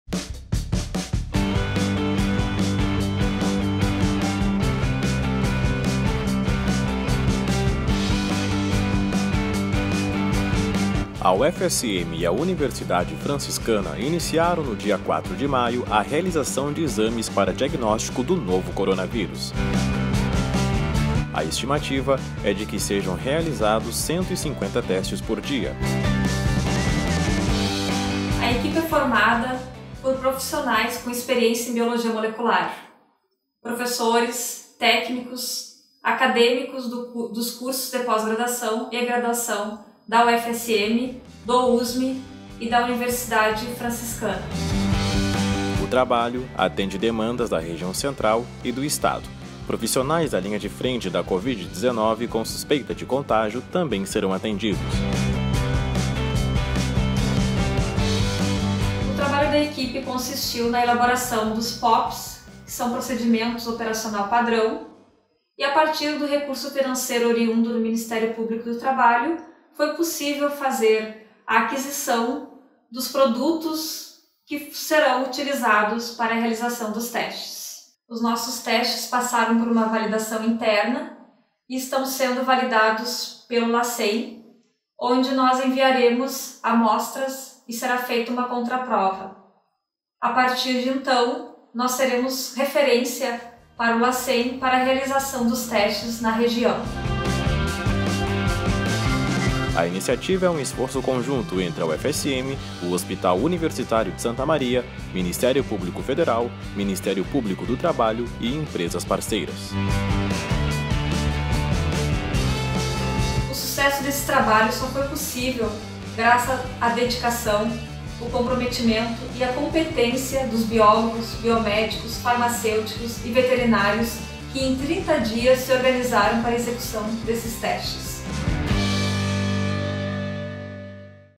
Testes para detecção da COVID-19 começam a ser feitos na UFSM
Uma força tarefa liderada pela UFSM está realizando testes para a detecção do novo coronavírus. O trabalho, que atende demandas da região central e do Estado, também é destinado aos profissionais da linha de frente da COVID-19 com suspeita de contágio.